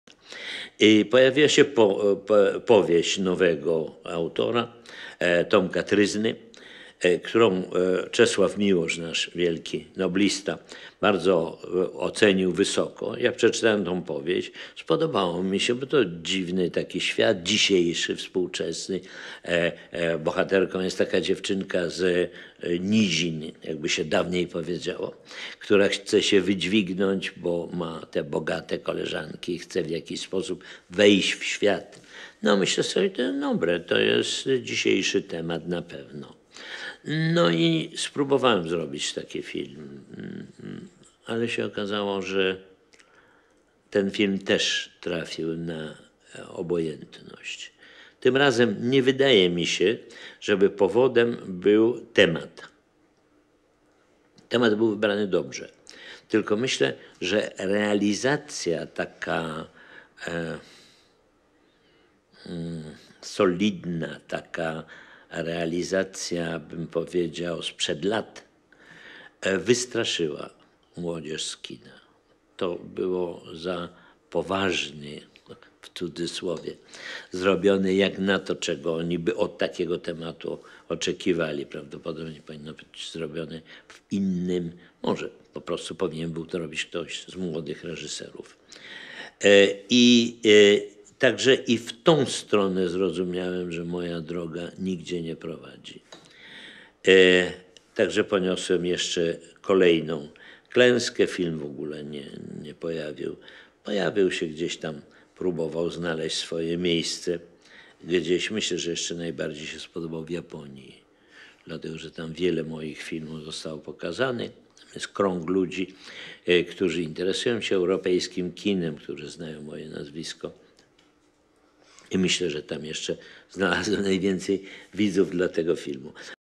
Andrzej Wajda - 'Miss Nobody' (176/222)
Polish film director Andrzej Wajda (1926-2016), whose début films portrayed the horror of the German occupation of Poland, won awards at Cannes which established his reputation as storyteller and commentator on Polish history. He also served on the national Senate from 1989-91. [Listener: Jacek Petrycki]

TRANSCRIPT: A story appeared by a new author, Tomek Tryzna, which our great Nobel prize winner Czesław Miłosz rated very highly. I read it and liked it because it was set in an odd, contemporary world. The heroine is a girl from the lower spheres, as they used to be called, who wants to rise above her beginnings because she has wealthy friends and she wants to make it in the world somehow. Well, I thought to myself, this is good, this is definitely a topic for today. So I tried to make this film but in the end it was received with indifference. This time, I don't think the topic was the reason. The topic was well chosen but I think that the way the film was made, so thoroughly in what I'd call an old-fashioned way, scared off the young audiences. It was made in a way that was 'too serious' compared with what they would have expected of such a topic. It probably should have been made in a different... perhaps it should have been made by a younger film director. So I realised that my way wasn't going to take me in that direction, either. So it was another failure for me, the film wasn't even released, or it was released somewhere and tried to find a place for itself. I think it was most popular in Japan because a lot of my films were shown over there. There's a group of people there who are interested in European cinema. They know my name and I think that's where I found the biggest audience for this film.